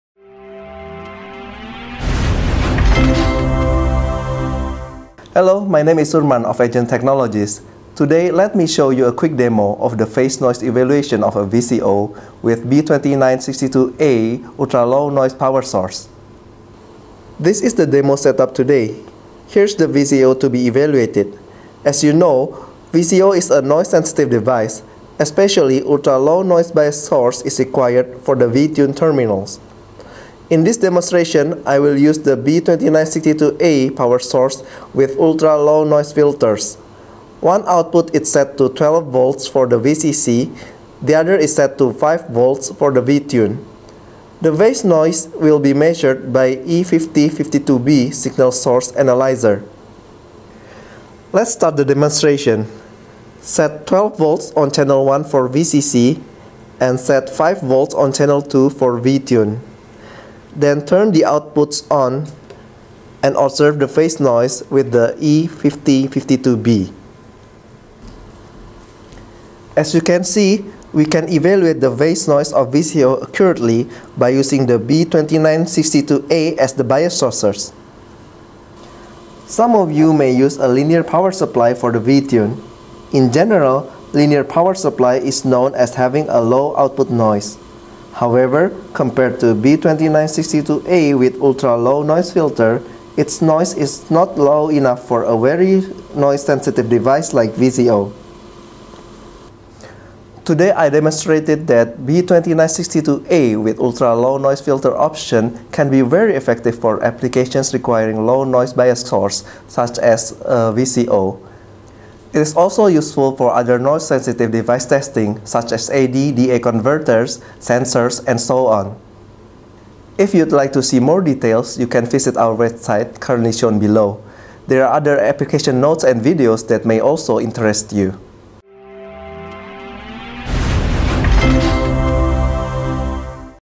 Phase noise evaluation of VCO using stand alone low noise power supply - B2960 - BEMT#13
[Closed Caption available] The VCO (Voltage Controlled Oscillator) is well known as noise sensitive device. Its output signal frequency is controlled by the DC voltage at the tuning port and the noise on Vtune directly impacts the signal frequency. In this video, a standalone power supply is introduced as effective solution for VCO bias source. This is also effective for other noise sensitive devices such as AD/DA converters, sensors and so on.